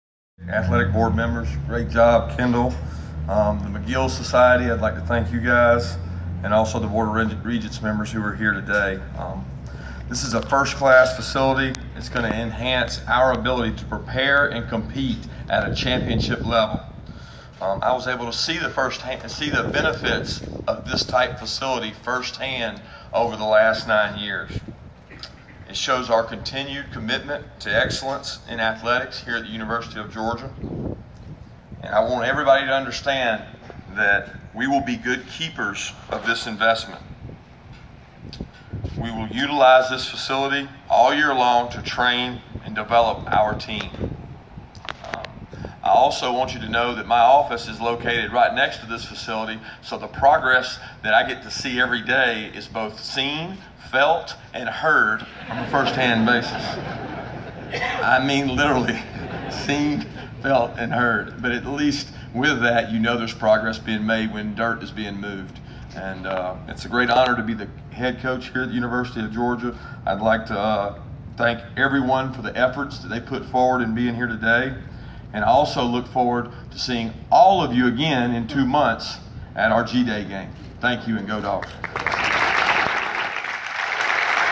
Kirby Smart speaks at UGA's groundbreaking ceremony for IPF
Smart addresses crowd